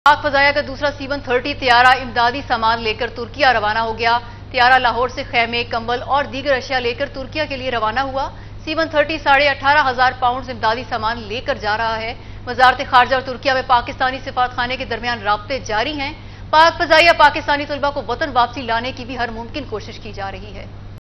The second C130 plane of Pakistan Air Force left for Turkey with relief supplies, Spokesman
پاک فضائیہ کا دوسرا سی ون تھرٹی طیارہ امدادی سامان لے کر ترکیہ روانہ ،ترجمان